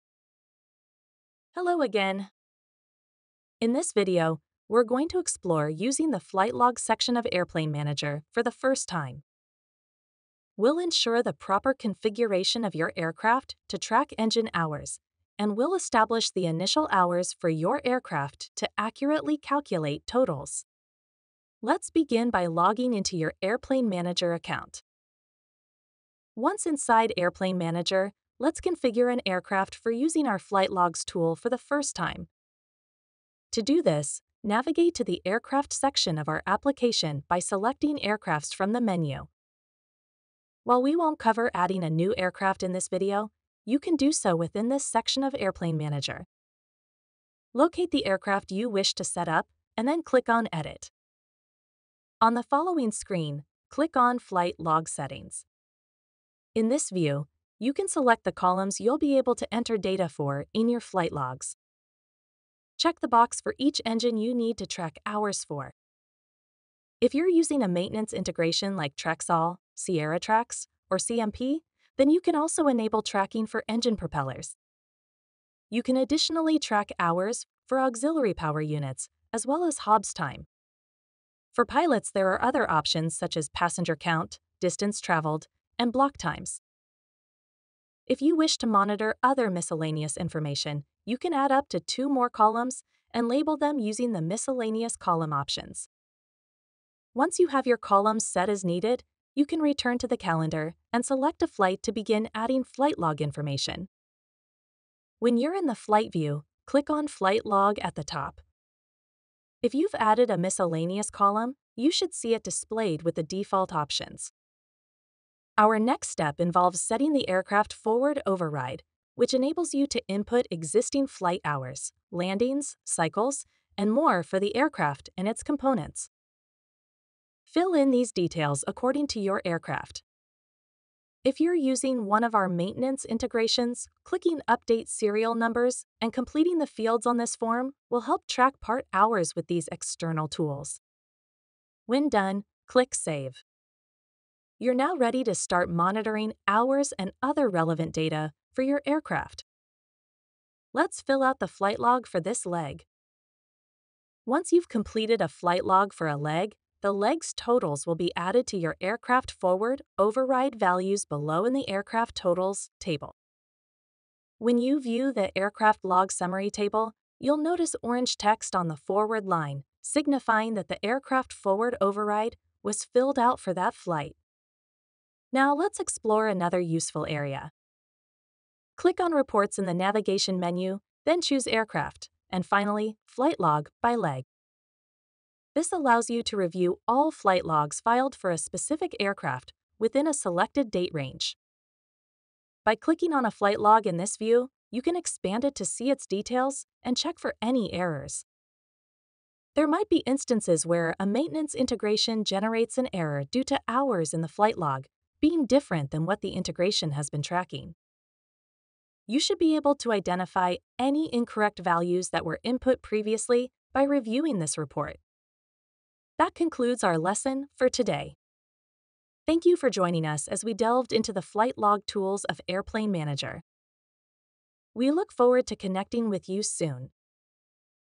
Mastering Flight Logs: Your Comprehensive Guide to Airplane Manager's Flight Log Feature
Unlock the full potential of Airplane Manager's Flight Logs in this detailed walkthrough.

Learn how to effortlessly configure aircraft for precise flight log data, set initial hours with the Aircraft Forward Override feature, and access comprehensive hour totals.

Dive into the Reports area to view all submitted flight log information.

Streamline your flight data management with Airplane Manager's intuitive Flight Logs feature.